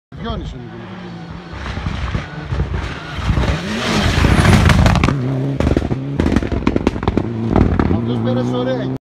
MIDER - Farmaki Maria Mitsubishi 8 - Rally Sprint Korinthou
Ραλλυ Σπριντ Κορίνθου 2019 Mider Kirkos racing